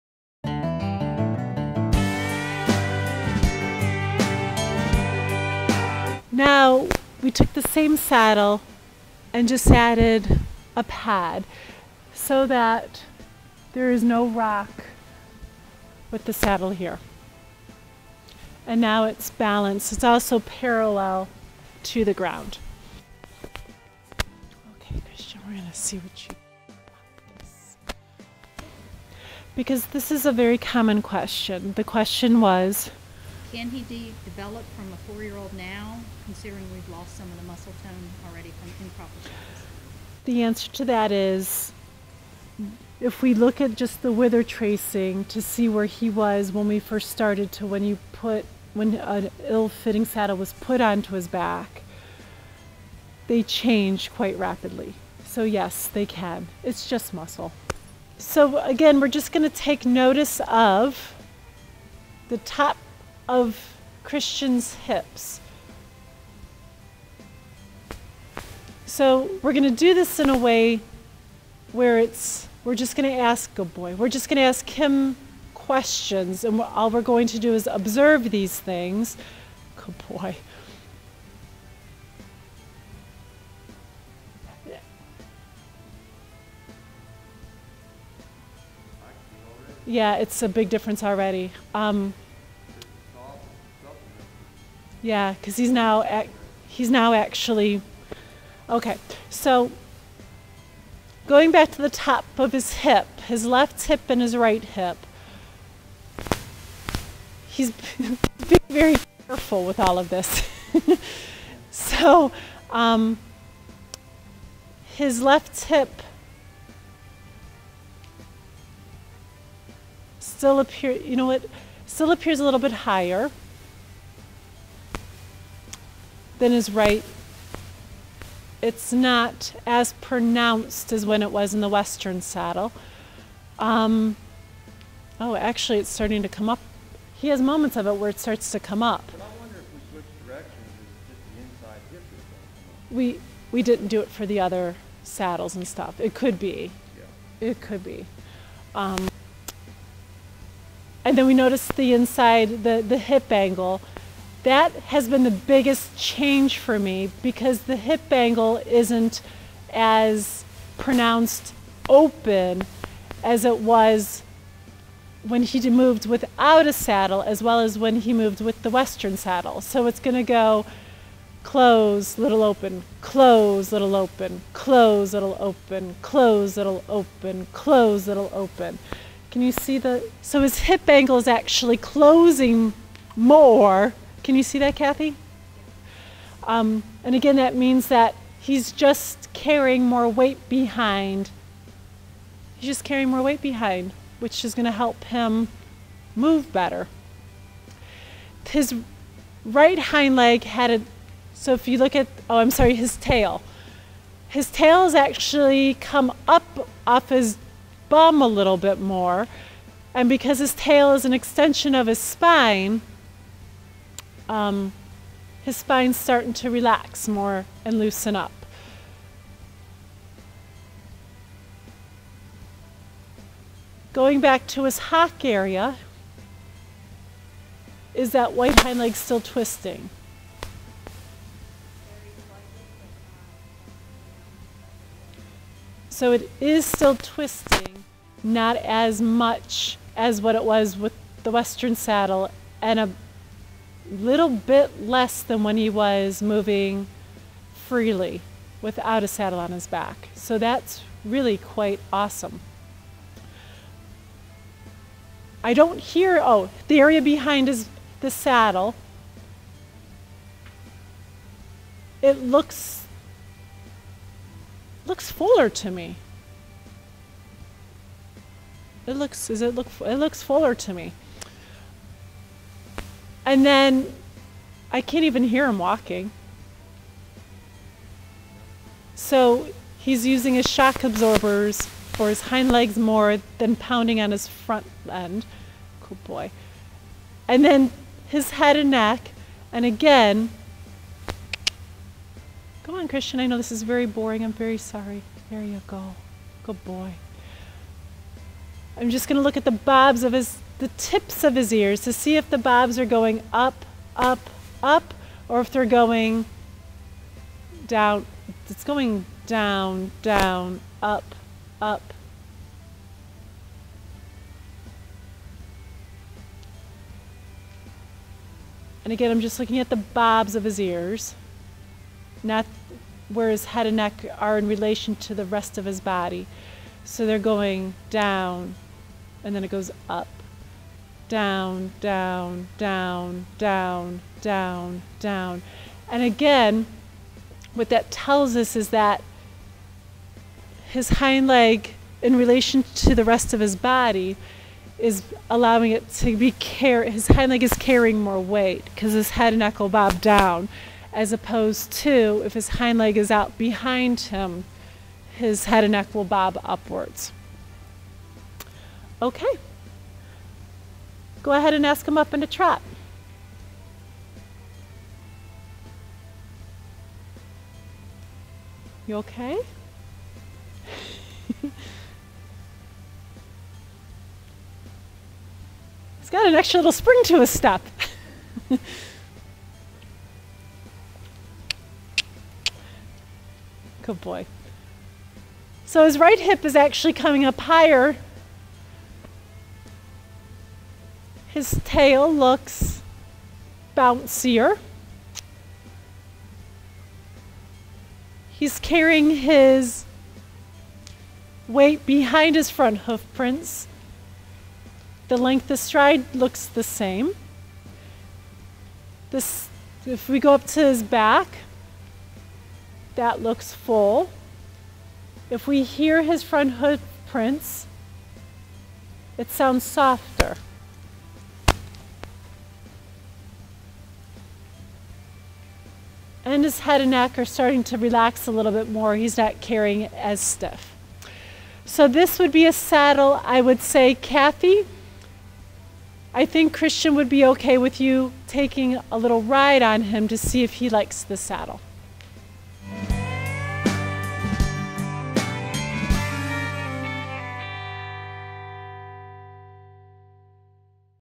Saddle Fitting - Part 8 (Christian)
The team tries an English saddle and analyzes the difference on Christian with the Western saddle.Visit Handyhorseman.com for many more videos